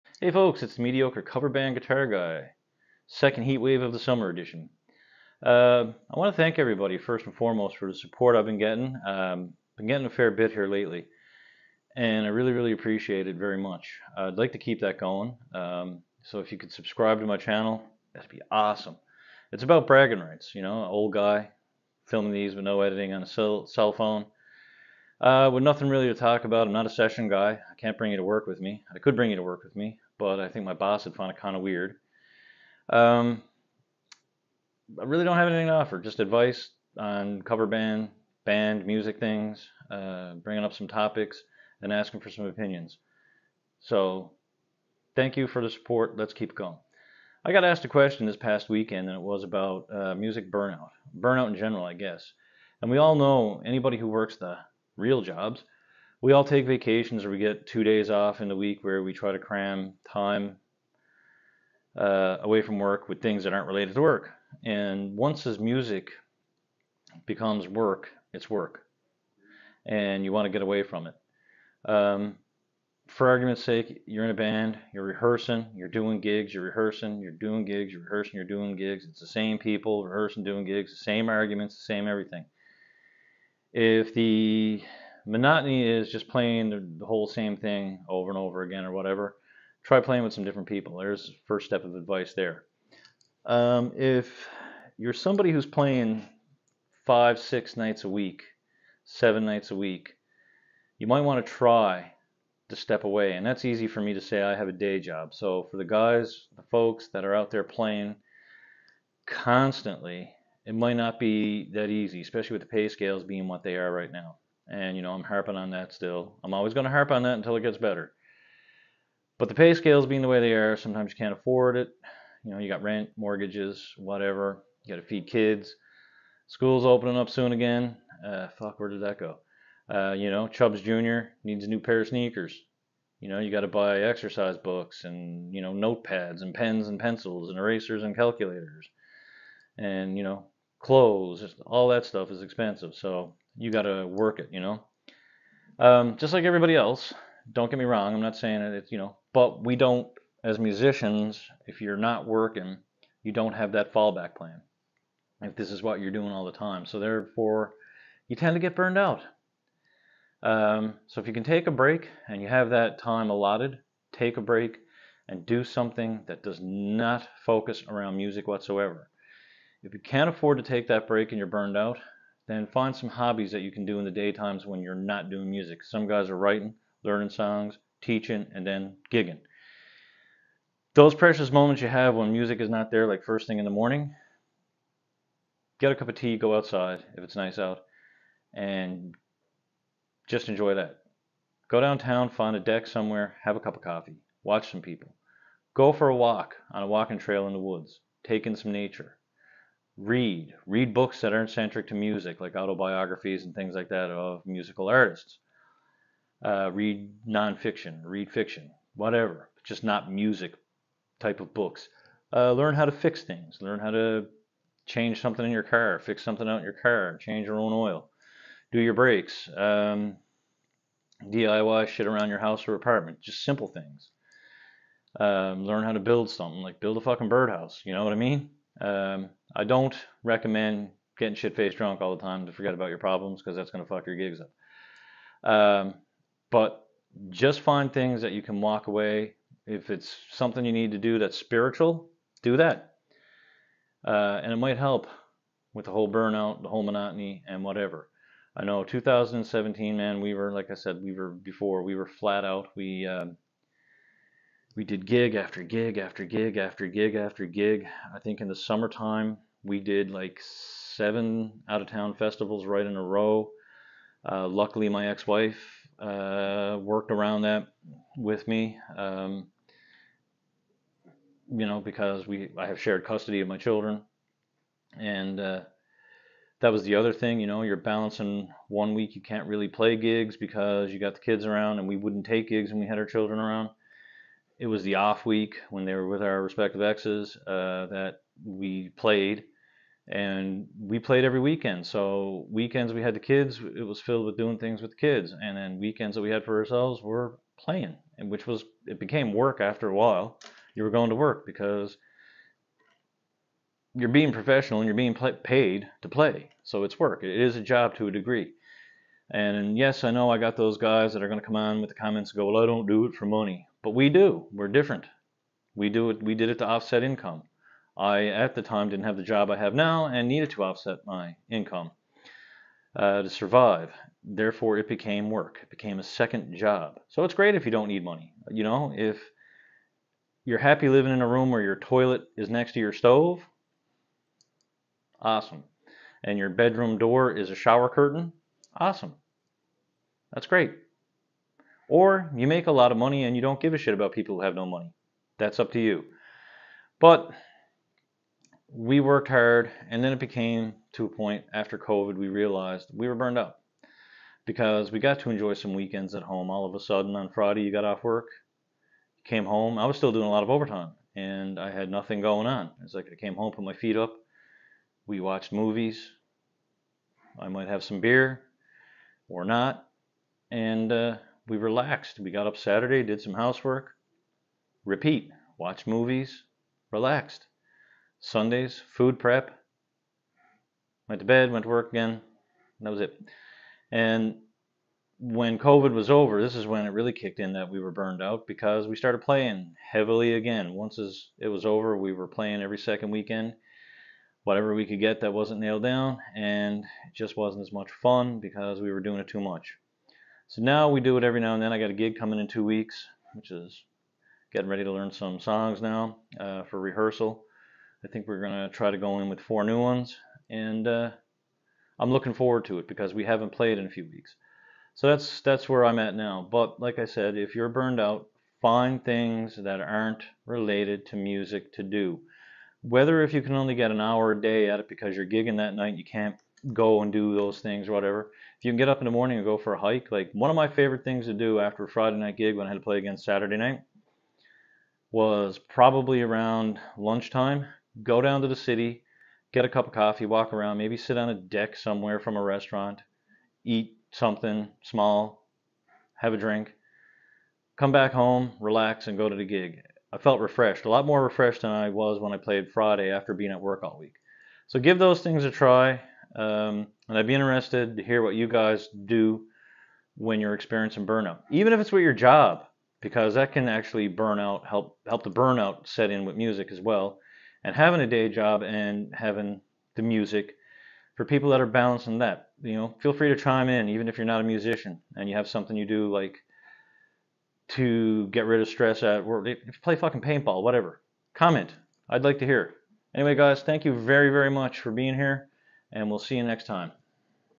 Dealing with being burned out.
Too much of a good thing can take a toll on you after a while. here are some things you can try when it happens to you.